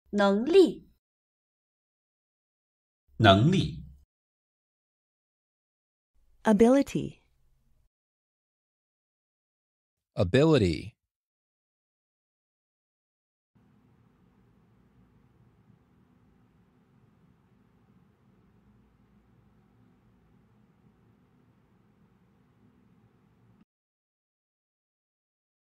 How to say "Ability" in Chinese (Traditional)
Male Voice:
能力

Female Voice:
能力

Male Voice Translation:
Ability

Female Voice Translation:
Ability